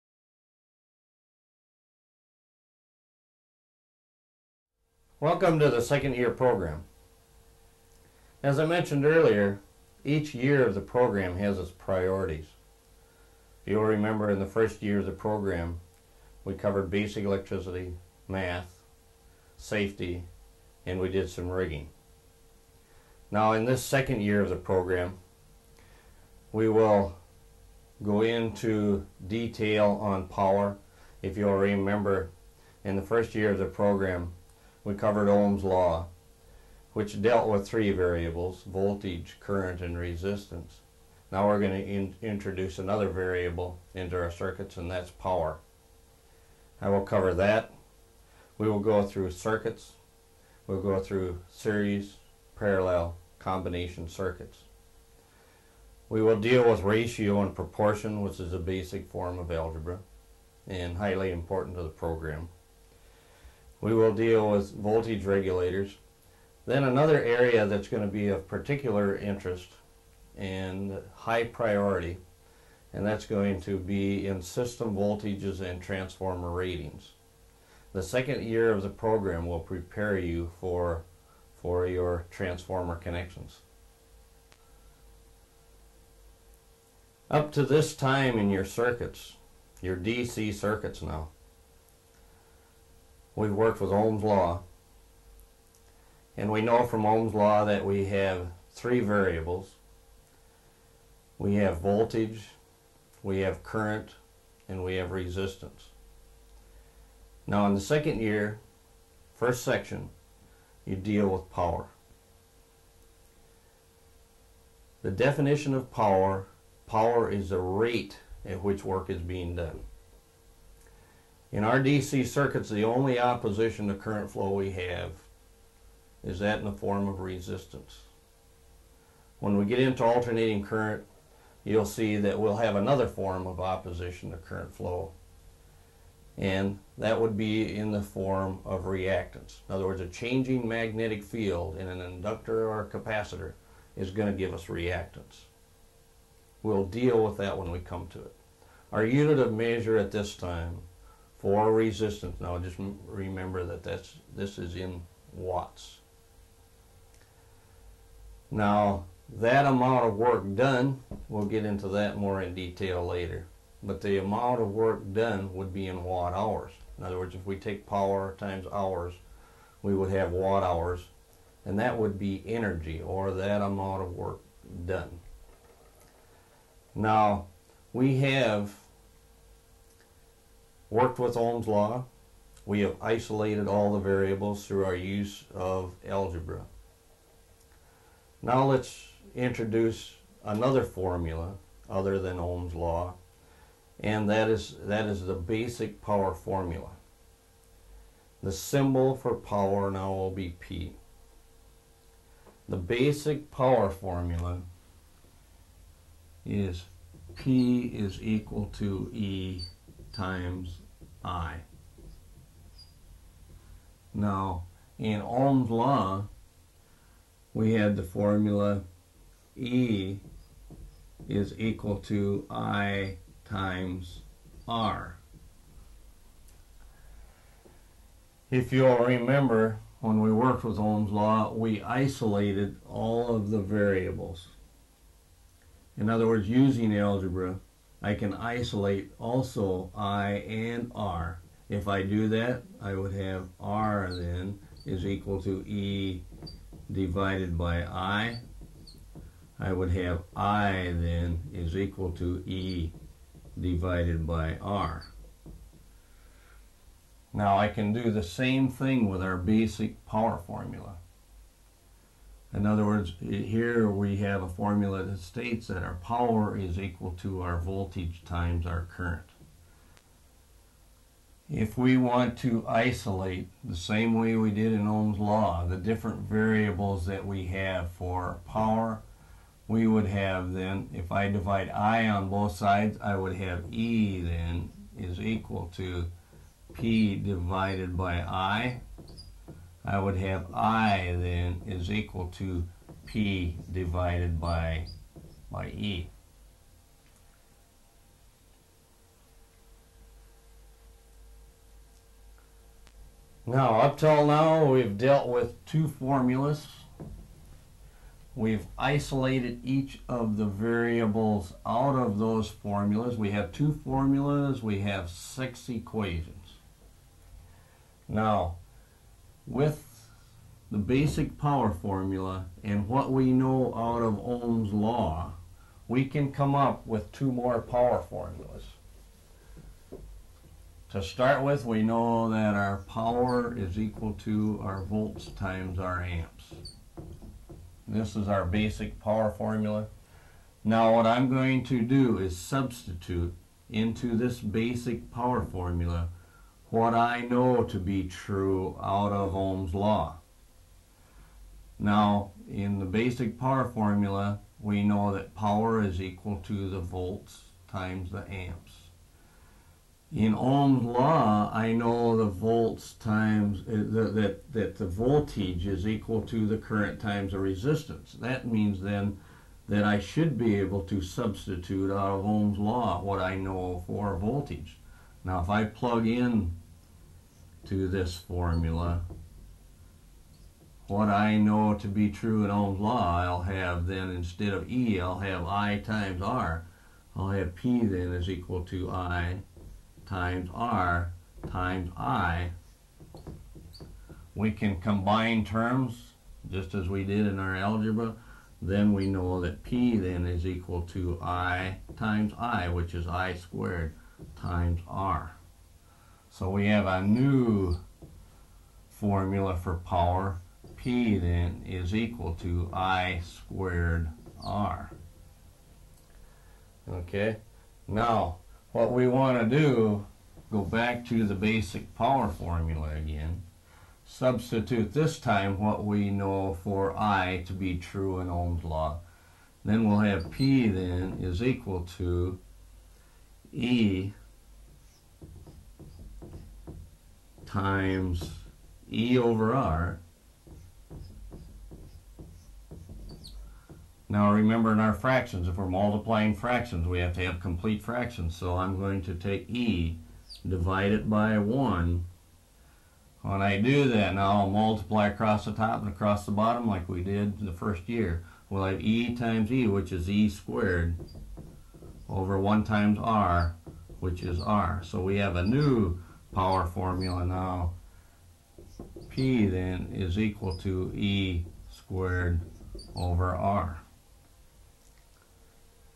Power Part 1
Dennis Merchant video on electric power calculations.  They go with second year of the Merchant Job Training & Safety Program basic electricity units.